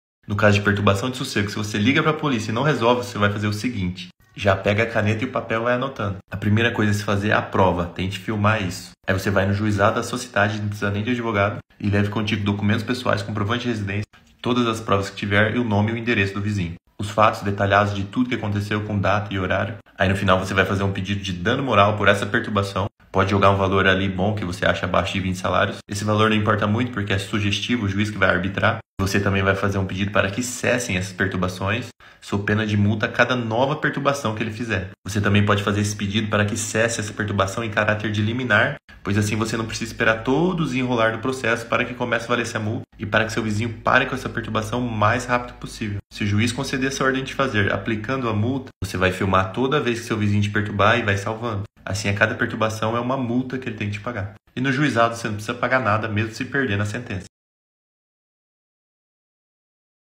PERTURBAÇÃO DE SOSSEGO, O QUE FAZER? SAIBA O QUE FAZER QUANDO A POLÍCIA NÃO RESOLVE
Saiba o que fazer quando seu vizinho perturbar seu sossego com excesso de barulho ou som alto.